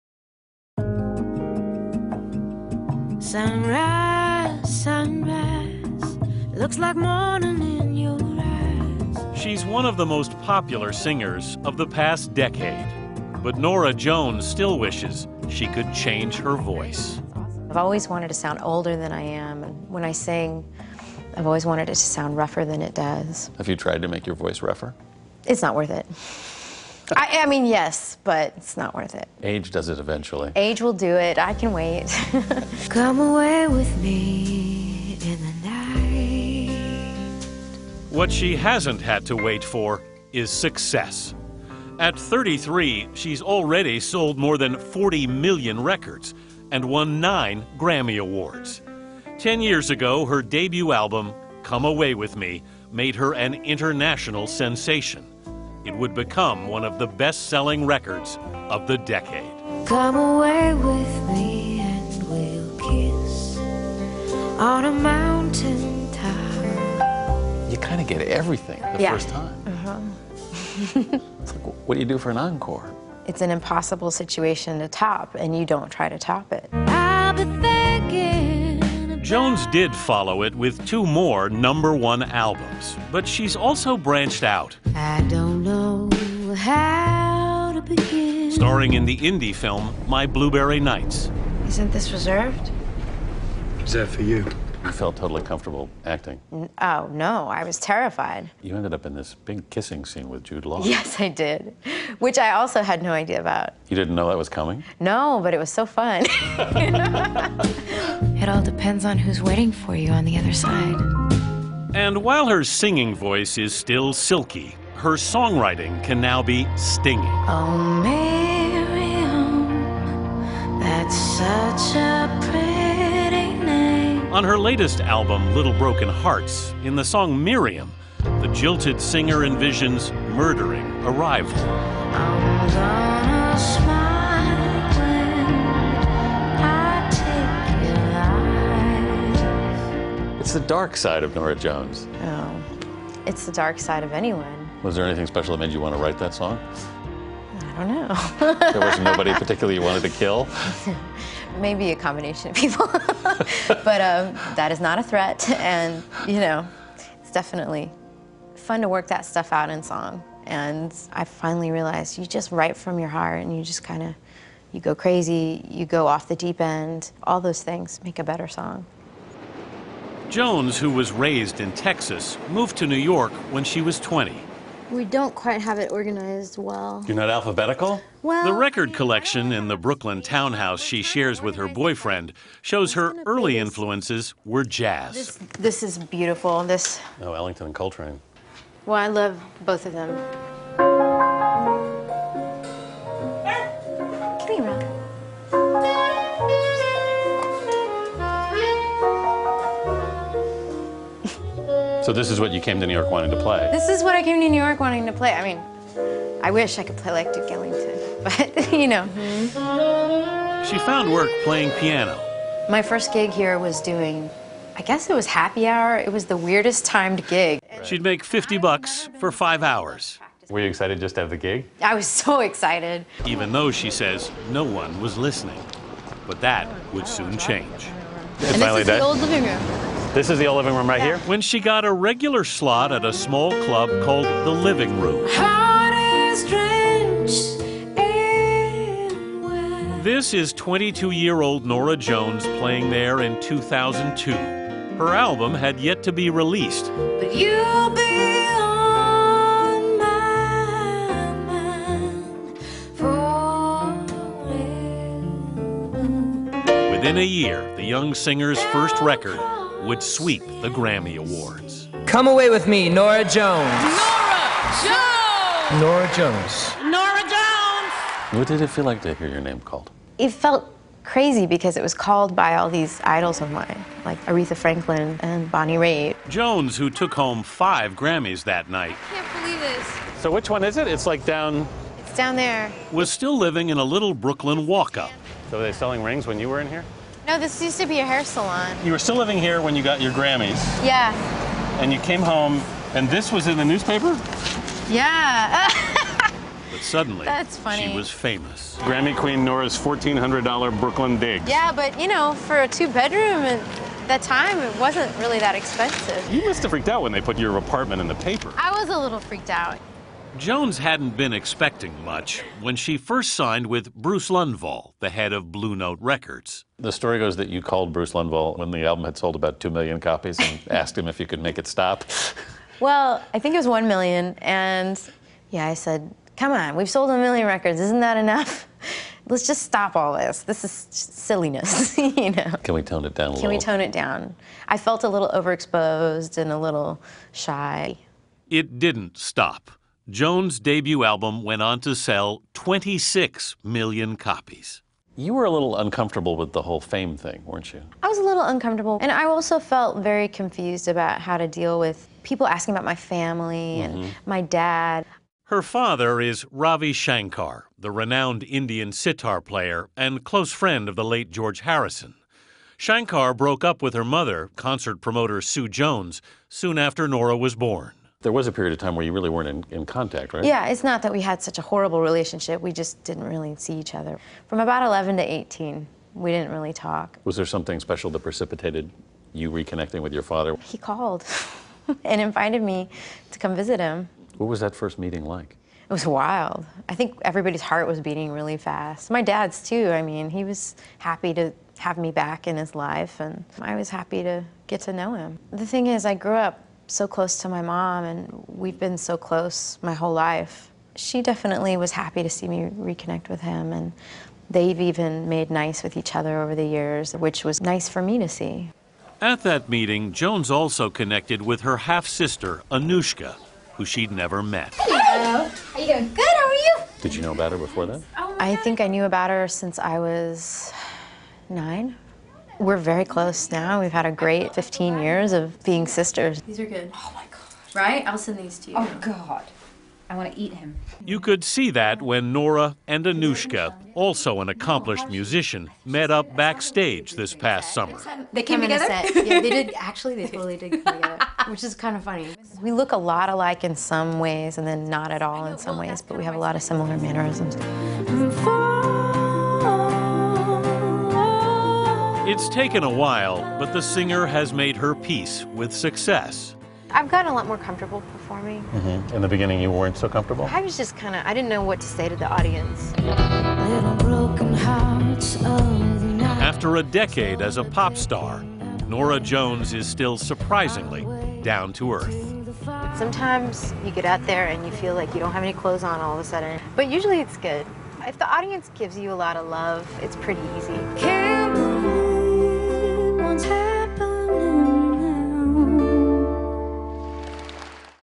The life and times of Norah Jones
Norah Jones has had a prolific career, selling 26 million copies of her first album alone. Anthony Mason profiles her music, her uneasiness with fame, and her next project.